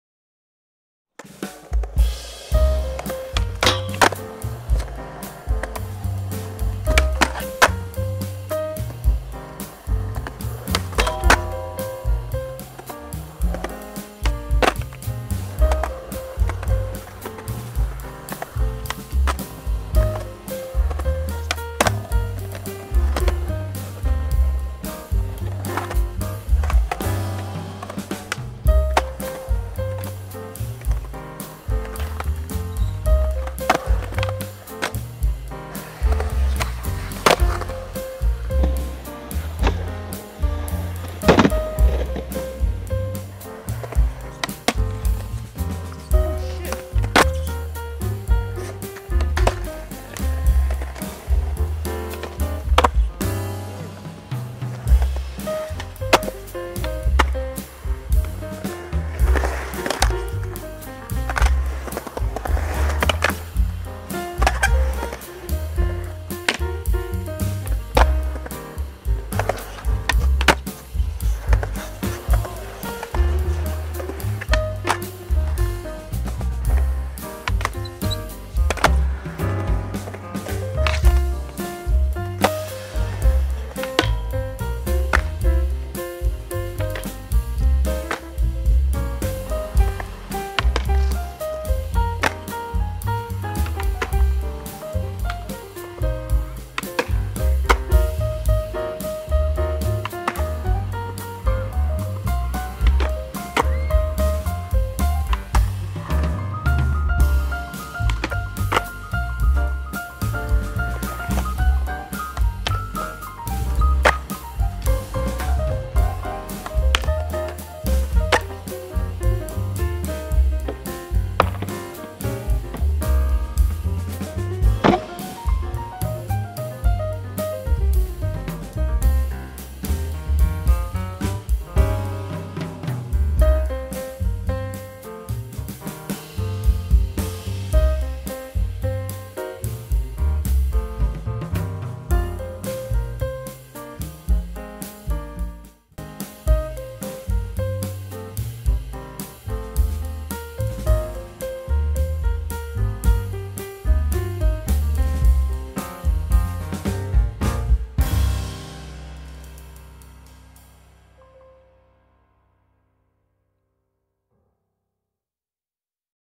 San Diego's Hidden Skatepark
Here's Calvin  DeBruler, Dominic Windey, Colin Patnaude and Falco Baltys skateboarding at the Del Sur Skatepark in San Diego, CA.